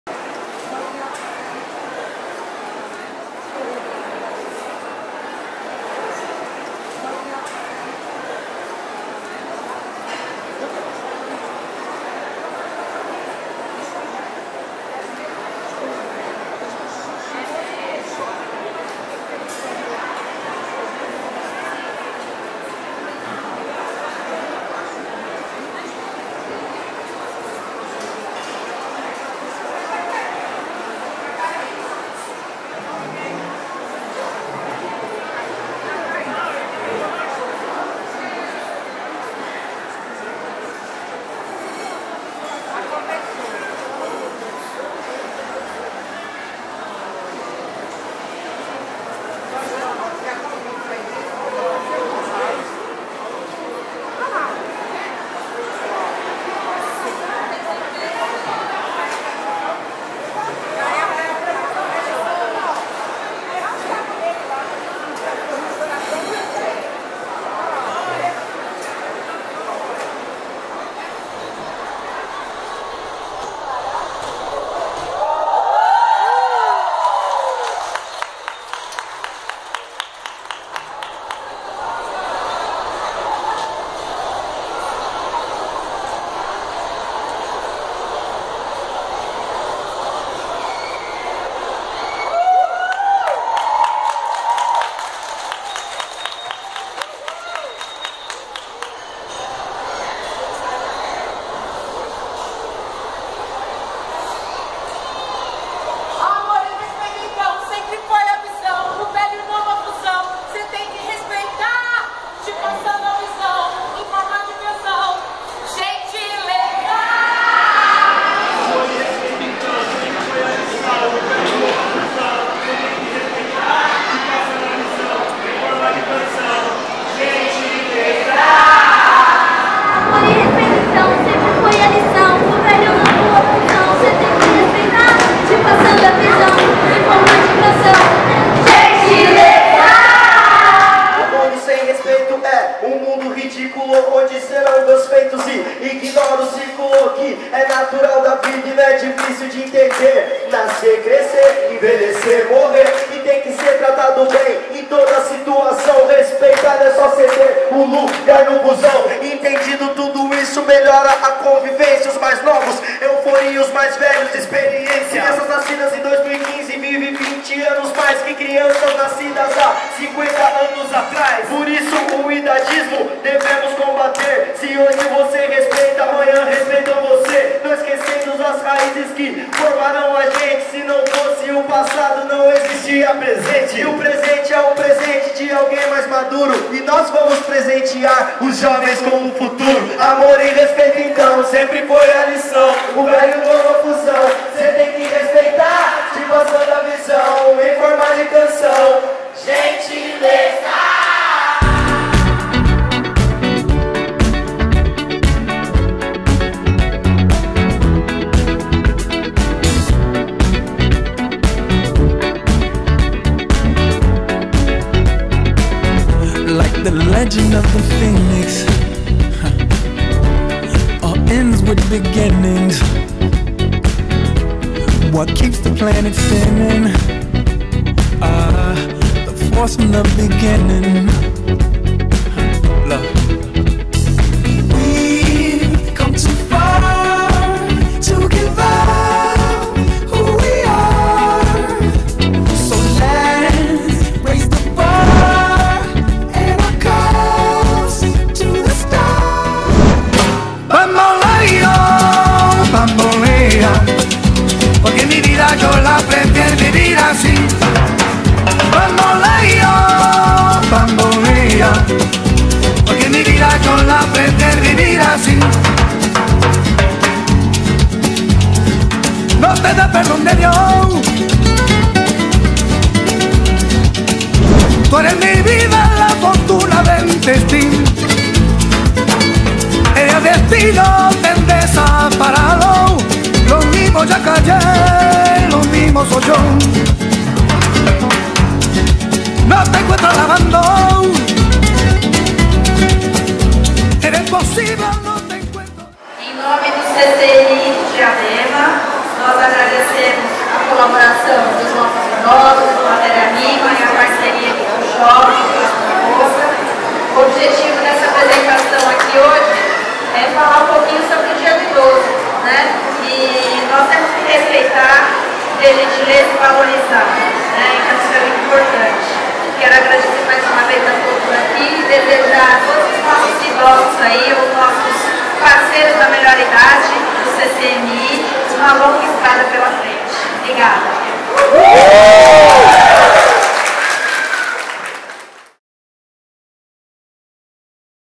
Matéria Rima-FLASHMOB DIA DO IDOSO-SHOPPING PRAÇA DA MOÇA -01/10/2017-DIADEMA/SP.
Há alguns meses fomos procurados pelo Thiago, do Centro de Convivência da Melhor Idade (CCMI), para escrevermos um rap sobre o Dia do Idoso. Na ocasião, ele nos deu uma bibliografia sobre um assunto muito importante, o Idadismo. Não sabíamos o que seria isso. Então, fomos estudar. Idadismo vem do inglês “ageism” e é o nome que se dá à discriminação de quem é considerado “demasiado velho para”. A partir do que lemos e de nossas vivências, começamos a ponderar: Em que idade somos demasiado velhos? O que é “saber envelhecer”? O que sucede a quem resiste à norma? Como formar crianças e jovens que respeitem os mais velhos? Que não se espantem ao vê-los dançar, cantar, brincar, ser feliz? A gente se propôs a mobilizar outras pessoas pra essa causa. E aceitamos o desafio feito por Thiago e a primeira-dama Caroline Rocha Michels - Secretária da SASC (Secretaria de Assistência Social e Cidadania) em fazer um flasmob no Dia do Idoso, bem na hora do almoço, no shopping Praça da Moça. O resultado: por meio de performances de teatro, música e dança, Matéria Rima, idosos do Centro de Convivência e o público heterogêneo que estava na praça de alimentação interagiram, mostrando que assim sempre deve ser: respeito, amor, alegria, educação

Artista: Matéria Rima
Prod Musical: Dj Meio Kilo
Letra: Joul / Nicolas Mc / Joseane Silva

Você já reparou nos netos e avós?
A relação desses dois é laço, não é nó!
O mais velho: experiência, sabedoria
O mais novo: curiosidade, energia

Gerações diferentes que não se excluem
Passado, presente que no futuro evoluem
Trocam de lado pra enxergar a luz
Que conduz a magia traduz o recado
Assim caminham passos lentos e apressados
Dentro de um abraço sintonizados
O mais velho não é peso, não é fardo
Só um pouco diferente e precisa de cuidado
Gentileza gera gentileza também
A criança envelhece e quer ser tratada como tratou alguém
O efeito dessa empatia
Seres humanos refeitos pela alegria
Netos transformados pela sabedoria
tenta compreender tamanha perfeição
só netos e avós sabem construir essa relação
amor e  respeito, essa é a lição

O mundo sem respeito é um mundo ridículo
Onde “cê” não vê os feitos e ignora o ciclo que é o natural da vida
Não é difícil de entender: nascer, crescer, envelhecer, morrer
E tem que ser tratado bem em toda situação
Respeitar não é só ceder o lugar no busão
Entendido isso melhora a convivência
Os mais novos: euforia. Os mais velhos: experiência
Crianças nascidas em 2015 vivem 20 anos mais
que crianças nascidas há 50 anos atrás
Por isso o idadismo devemos combater
Se hoje você respeita, amanhã respeitam você
Não esquecendo as raízes que formaram a gente
Se não fosse o passado não existia presente
O presente é o presente de alguém mais maduro
E nós vamos presentear os jovens com o futuro

REFRÃO

Amor e respeito então sempre foi a lição
O velho e novo a fusão
“Cê” tem que respeitar
Te passando a visão em forma de canção
Gentileza!

Shopping Praça da Moça